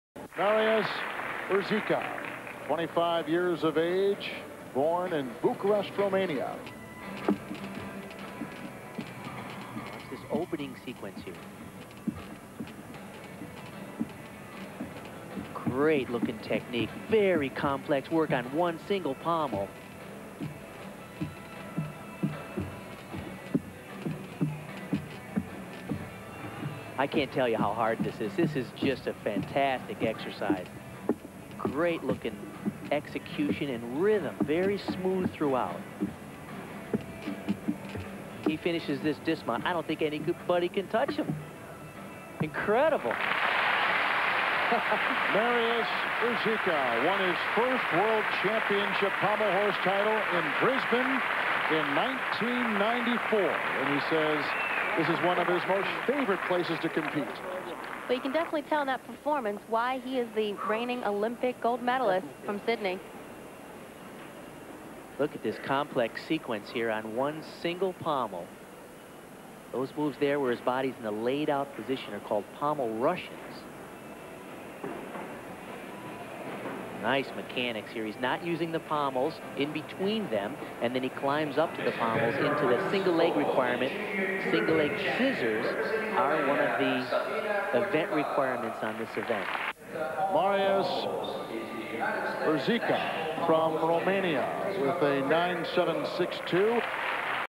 Marius URZICA (ROM) PH - 2001 Goodwill Games EF
Marius URZICA (ROM) - Pommel horse

'01 Goodwill Games

9.762
Rank : 1st